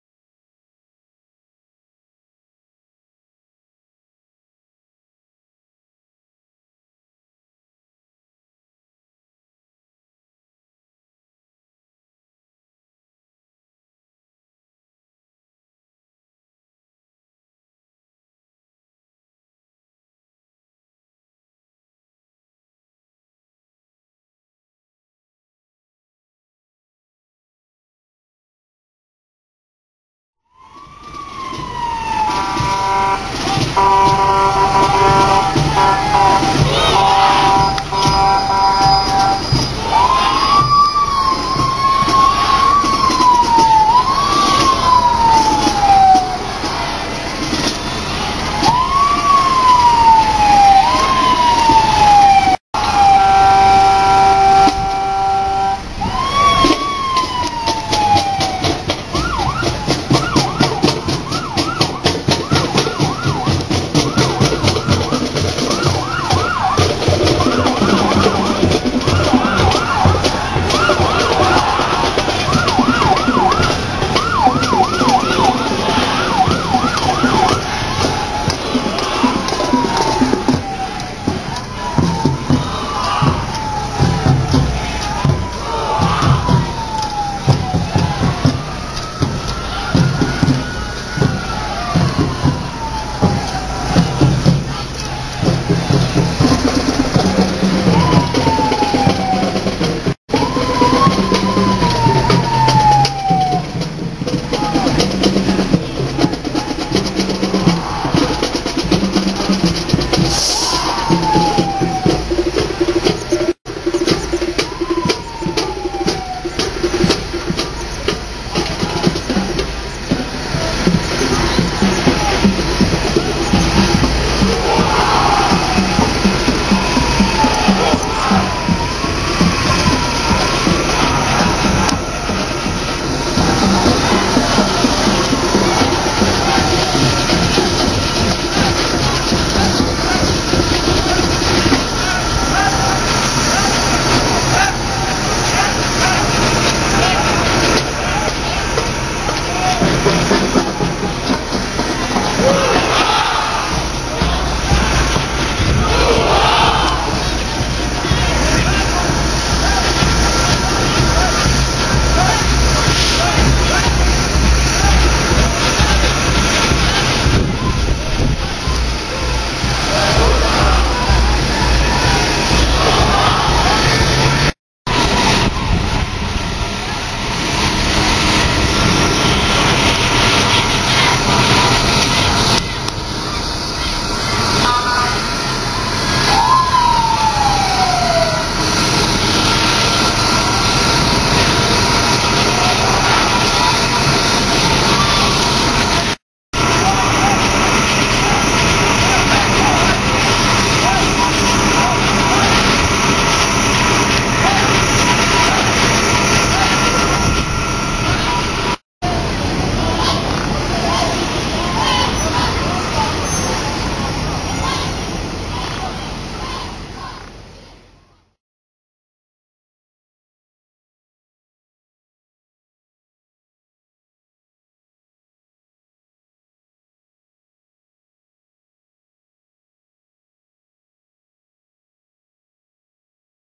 Go, Seaside Spartans!
Rally Parade by Seaside High School.  There's a lot of pride there, and I'm so proud of our future young men and women!